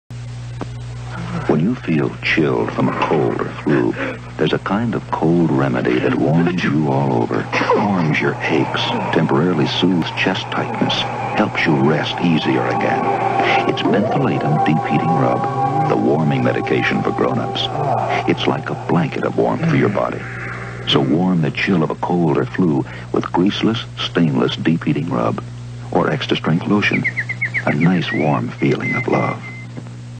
Menthalatum Commercial Ad 1974
Here's A Ad For Menthalatum's Deep Heating Rub.

BTW, I Used To Use It Alot When I Was A Kid.

Voiceover By ?